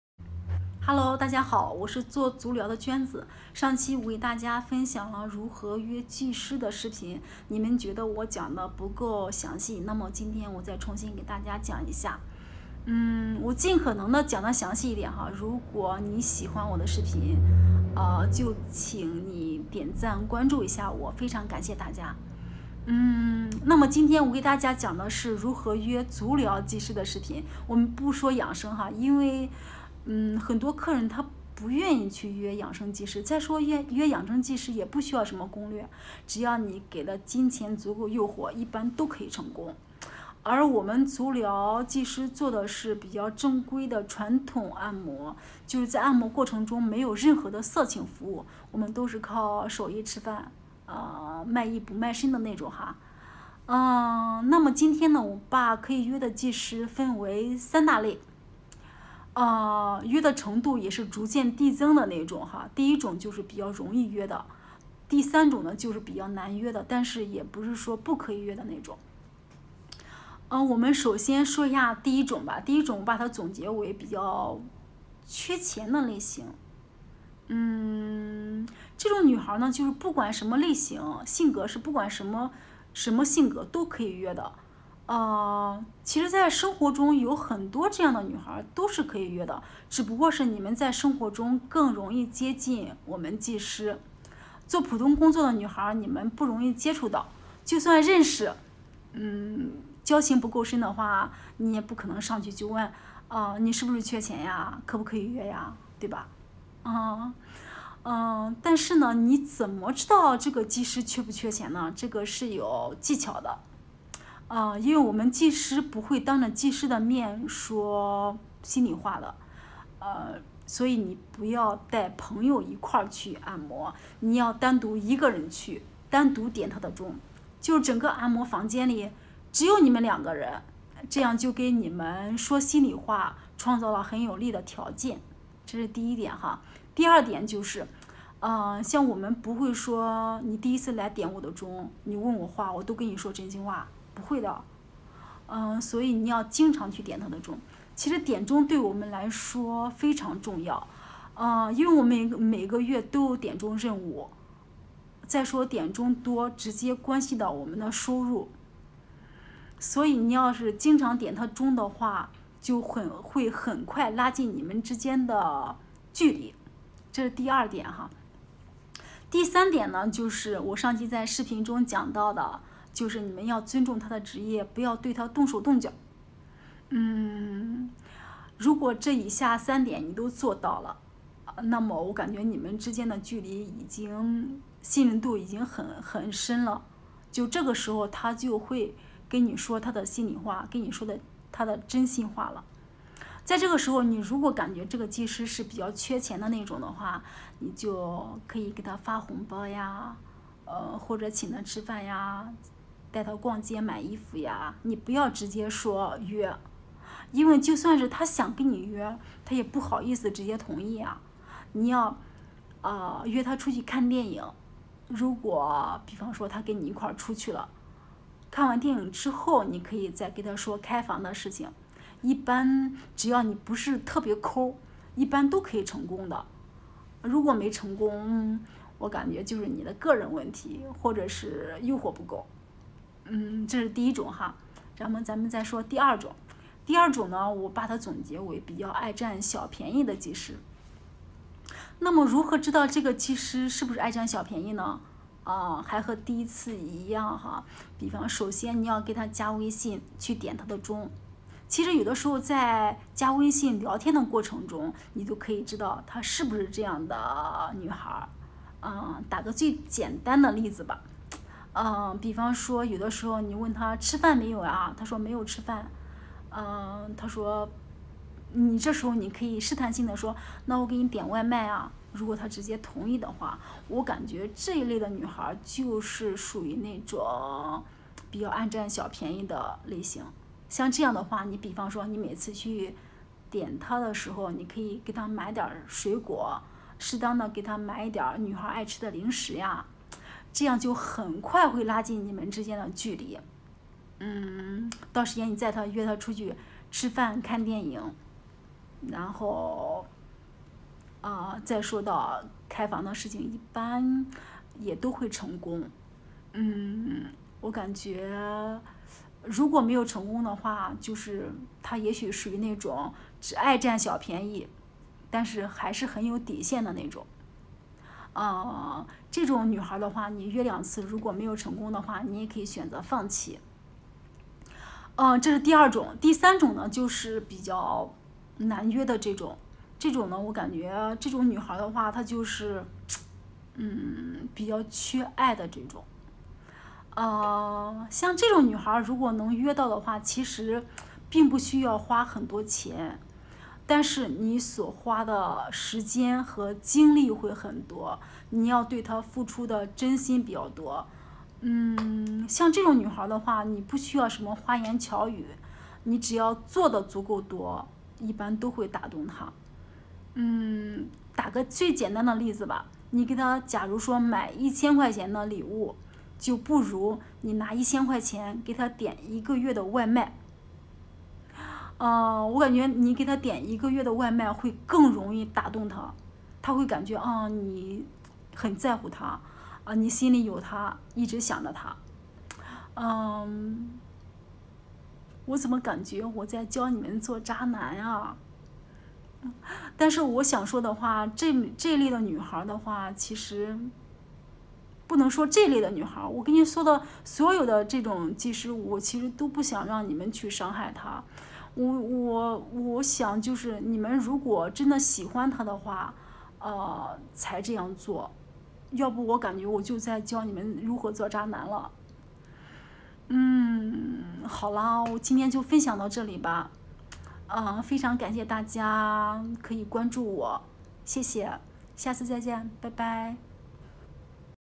怎么和足疗技师约会第二版！约会足疗技师最详细攻略，第三类足浴按摩技师最难成功
怎么和足疗技师约会？约会足疗技师最详细攻略，第三类最难成功